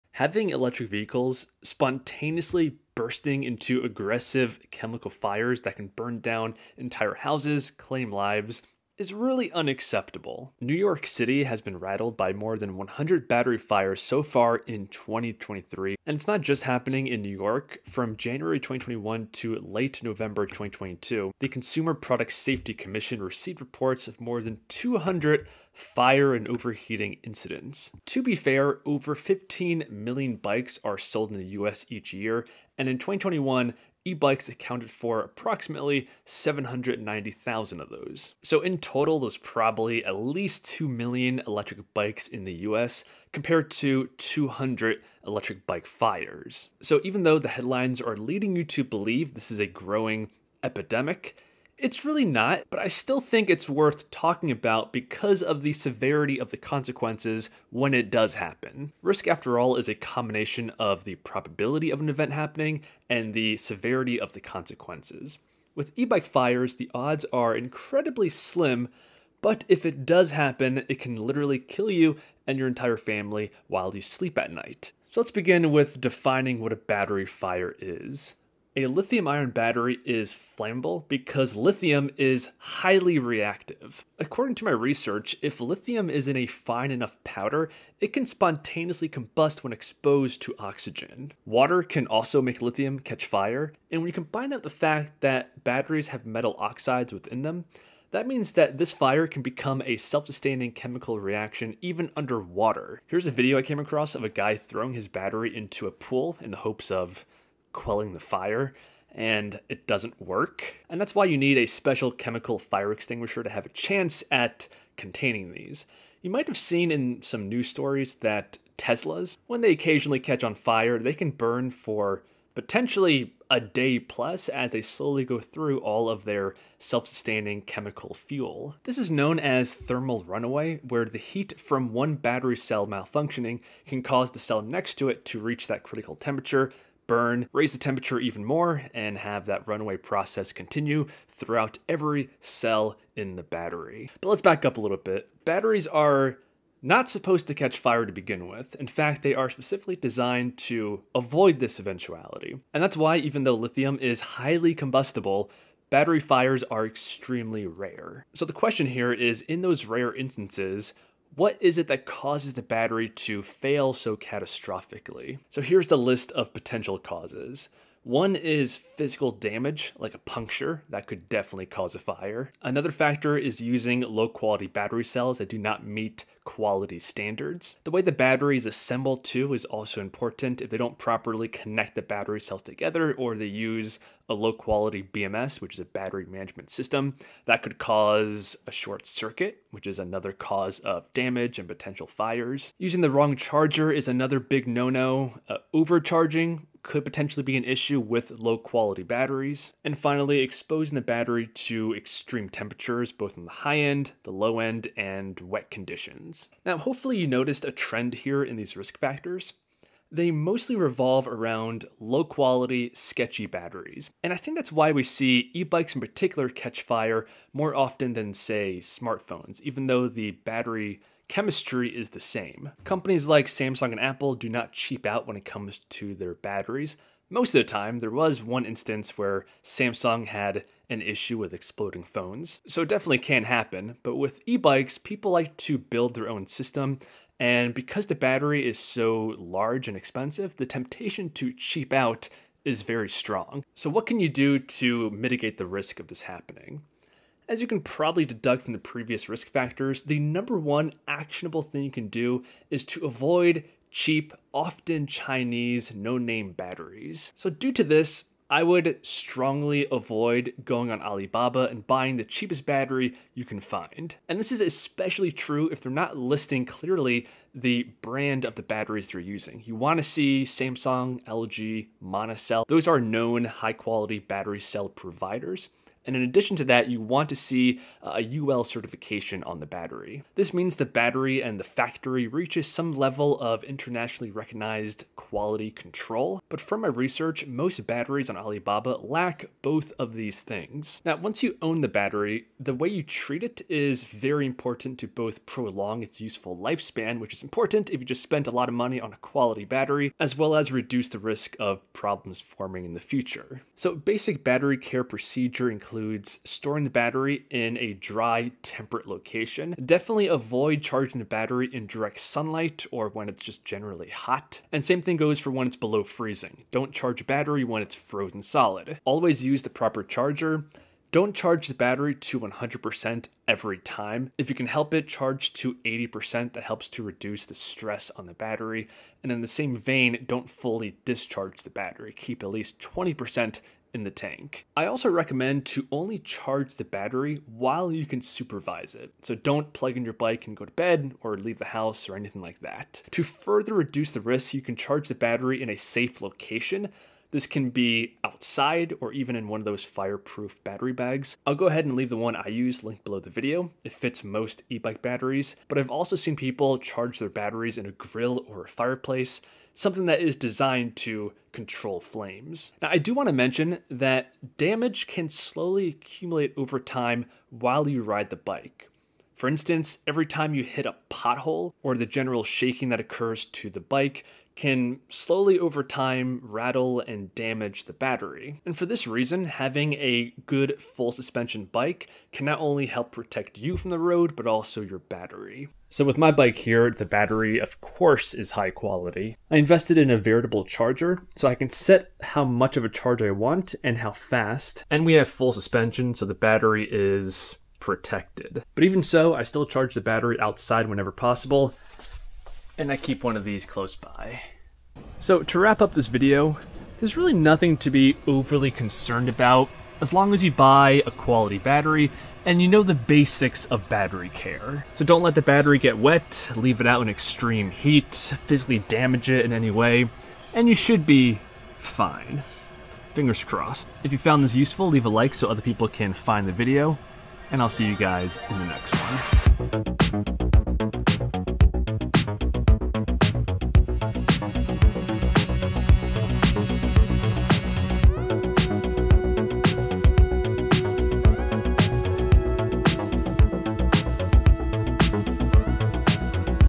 eBikes are Catching Fire and It’s a Problem
Electric vehicles catching fire is a big problem and needs to be resolved if we are going to rely more heavily on them in the future. This includes ebikes, electric scooters, electric cars, and other EV’s. Here I explain what causes battery fires and how you can minimize the risk.

Ebike fire battery bag

My riding gear:
Heated gloves